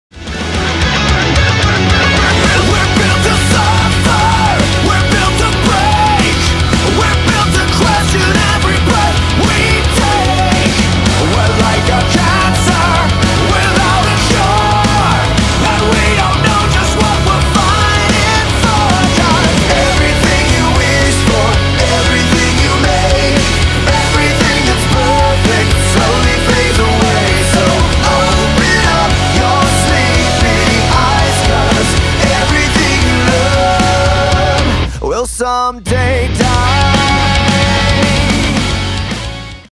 FREE SHOW! Cage9 + Bullets & Octane @ The Slidebar
Cage9, Pullman Standard opening for Bullets & Octane Friday, December 15th @ The Slidebar in Fullerton, CA.  FREE / 21+